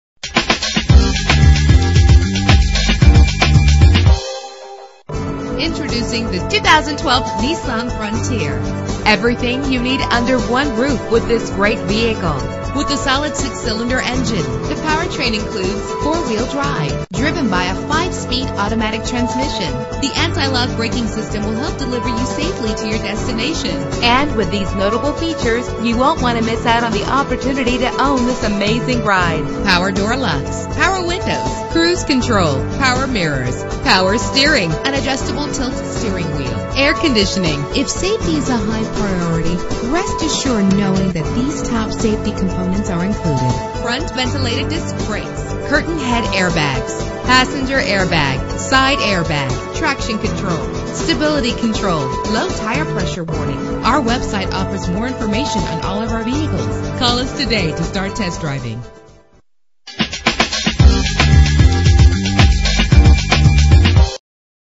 2012 Nissan Frontier - Nashua NH
Year: 2012
Make: Nissan
Model: Frontier
Trim: SV
Engine: 4.0 liter 6 cylinder 24 valve
Transmission: 5-Speed Automatic
Color: Blue Metallic
Mileage: 8
Address:
280-300 Amherst St